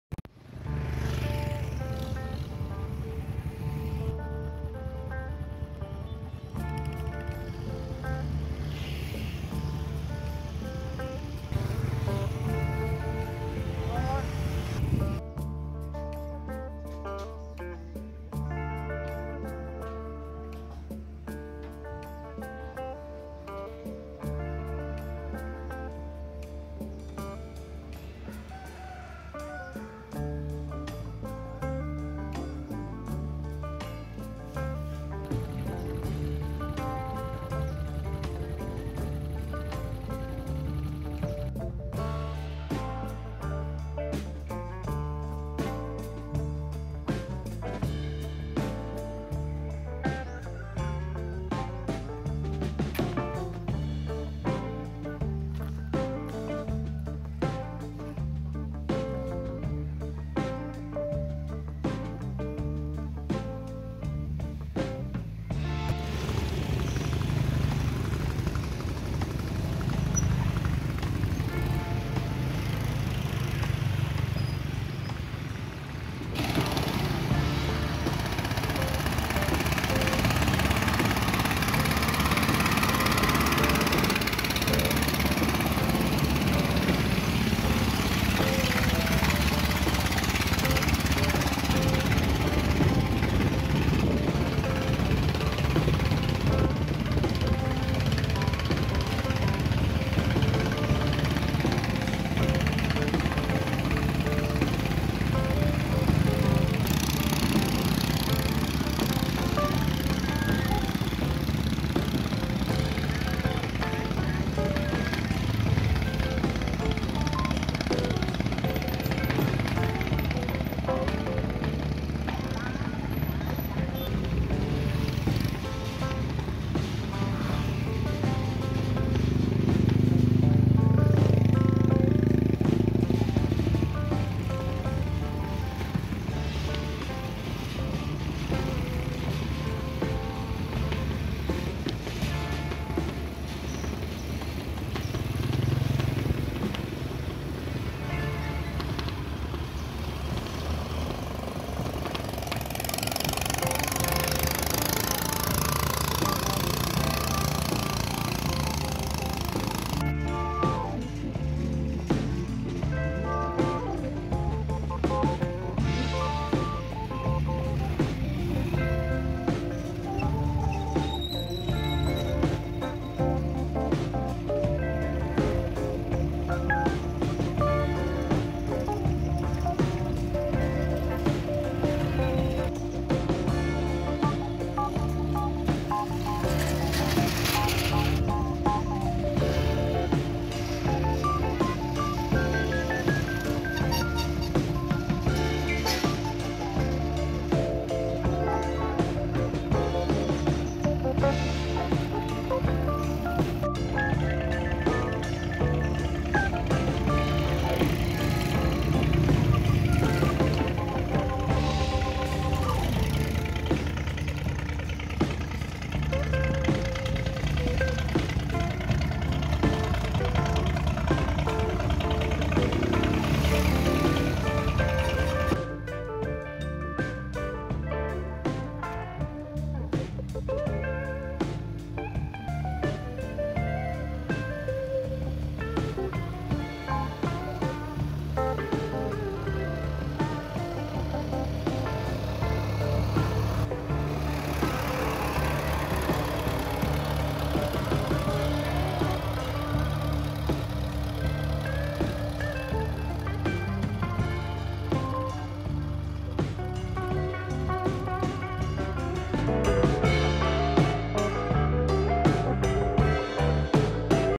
👉 GHE HÀNG TẤP NẬP, BỒNG BỀNH TRÊN SÔNG MIỆT THỨ - THỨ ①①❗
👉 MIỆT THỨ NHỘN NHỊP TRÊN SÔNG ĐẦU THÁNG ➌❓
      ➸ GHE HÀNG TẤP NẬP
           BỒNG BỀNH TRÊN SÔNG THỨ 11❗

      ① - Nằm giữa trung tâm Thị Trấn Thứ 11
             có  ➊khu vực dành riêng cho các ghe hàng
             khắp nơi dừng chân và buôn bán
             ➸ Cầu Kênh Hãng.

      XEM THÊM THÔNG TIN TỪ TRANG:
      ➸ FACEBOOOK @MIETTHUTHU11